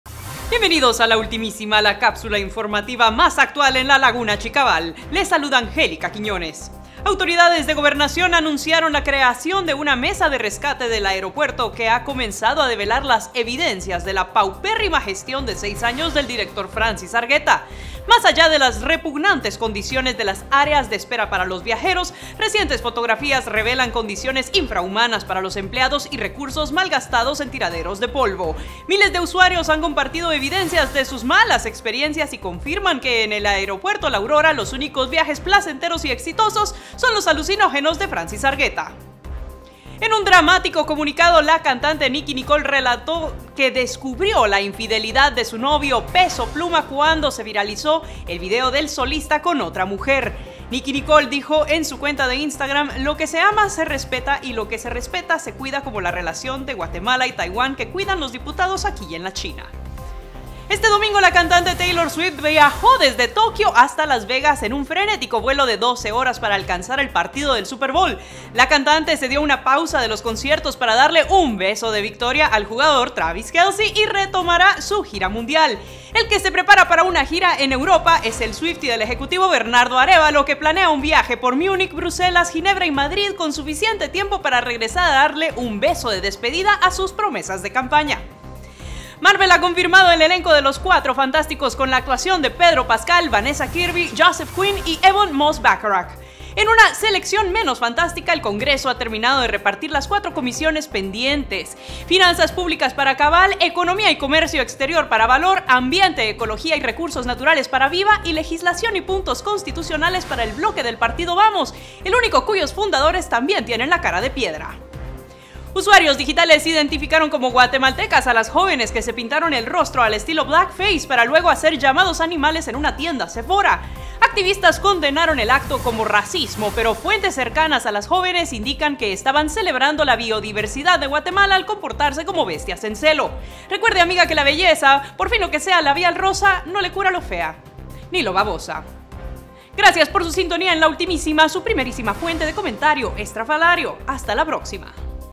"La Ultimísima"
Es la cápsula para actualizarte con lo más comentado y reposteado en las redes. En el episodio de hoy, celebramos la biodiversidad en Guatemala.